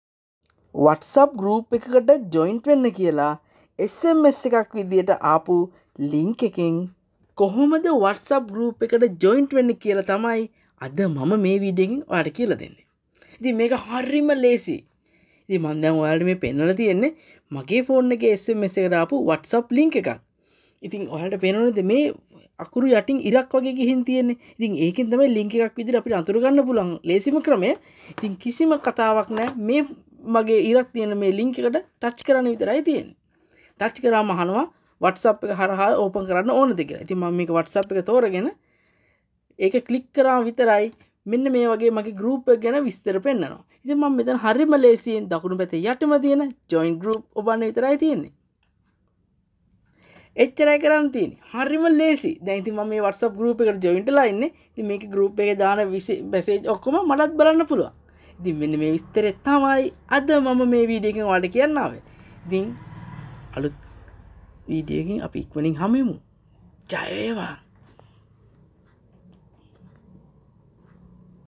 58 - ලින්ක් එකෙන් වට්ස් අප් ගෲප් එකට සෙට් වෙමු.
බොහෝ දෙනා අදහස් හුවමාරු කරගන්නා සමාජ මාධ්‍ය අතරින් වට්ස් අප් මාධ්‍යයට හිමිවන්නේ සුවිශේෂී ස්ථානයක්. මෙහිදී  තම අදහස් එකවර බොහෝ දෙනා අතර හුවමාරු කර ගැනීම සඳහා නිර්මාණය කරගත හැකි වට්ස් අප් කුඩා කණ්ඩායමකට තවත් කෙනෙක් එකතු කර ගැනීම සඳහා එම කණ්ඩායමට එකතු විය හැකි ලින්ක් යැවීම බොහෝ ප්‍රචලිත වී ඇත.
එලෙස අපට පැමිනෙන ලින්ක් මගින් වට්ස් අප් කණ්ඩායමකට එකතු වන ක්‍රම එය මෙම වීඩියෝව මගින් පැහැදිලි කර දෙනු ලබයි.